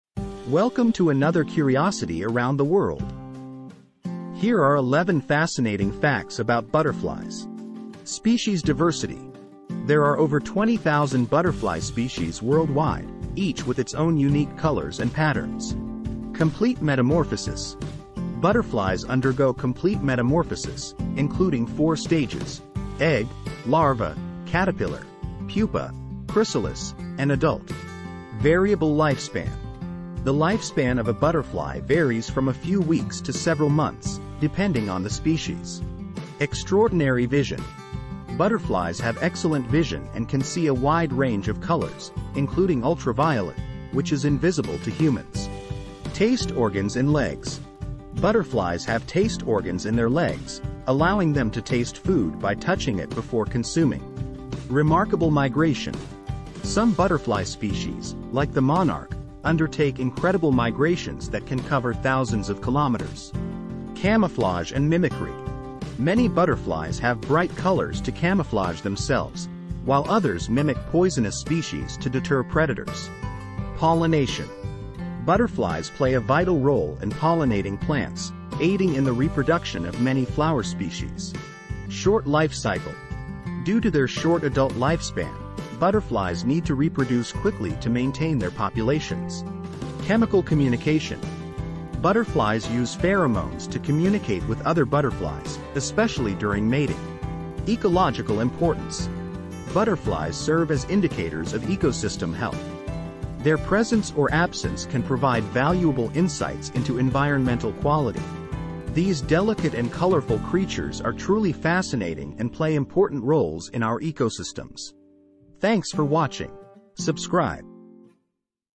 Discover the Enchanting World of Butterflies: 11 Fascinating Facts.
Explore the amazing world of butterflies in this video packed with fascinating facts and surprises. Learn about the life and behavior of these colorful creatures, from their incredible transformation in the pupal stage to their importance in plant pollination. Don't miss these 11 incredible butterfly facts!